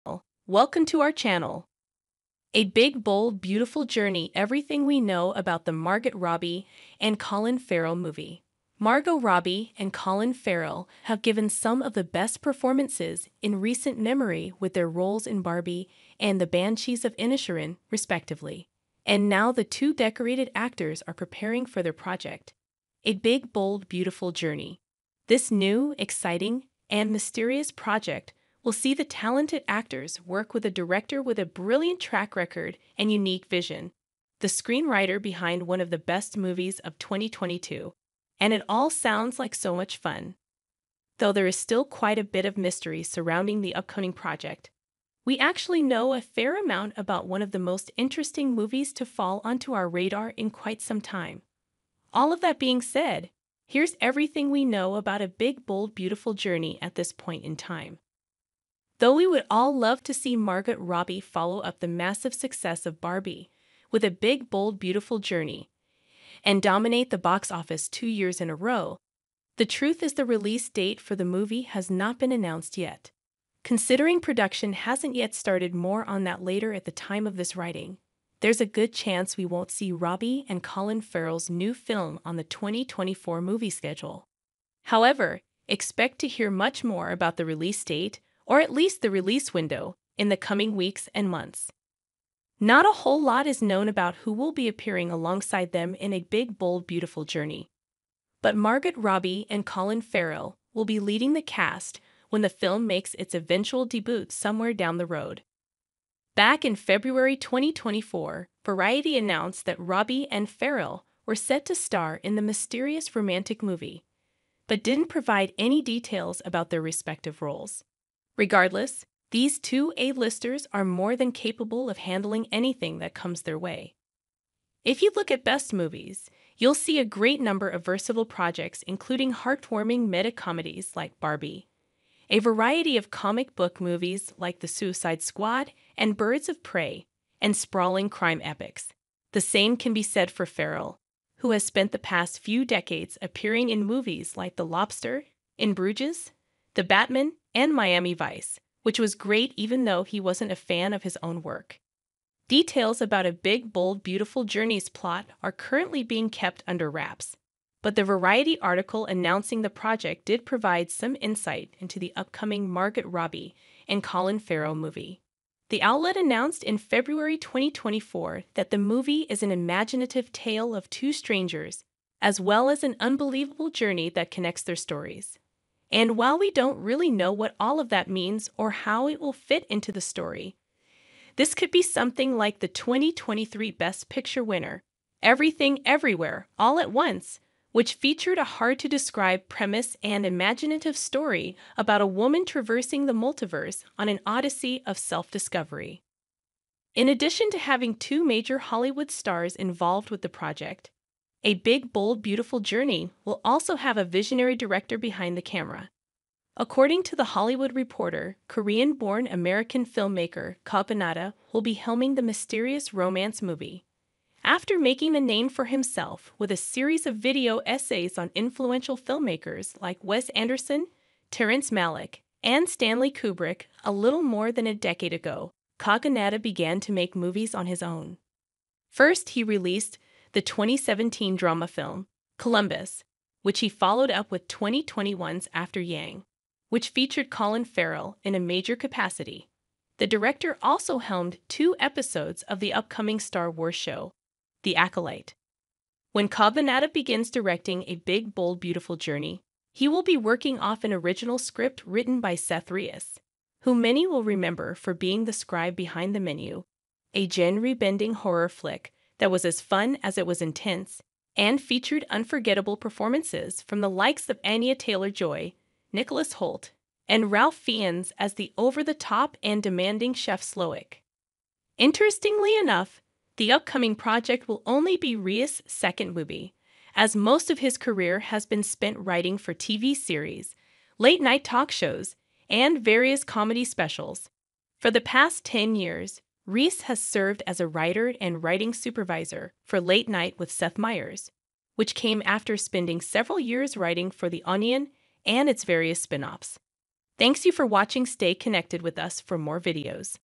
The Margot Robbie and Colin Farrell movie is a big, bold, and beautiful journey
Margot Robbie and Colin Farrell are set to star in a mysterious romantic film, A Big Bold Beautiful Journey. The film, directed by Korean-born American filmmaker Kogonada, is set to tell the story of two strangers on a journey that connects their stories. The film is expected to be released in 2024, with the release date and release window being unknown. The film is expected to be similar to the 2023 Best Picture winner, Everything Everywhere All at Once. The film is expected to be a thrilling and intriguing project.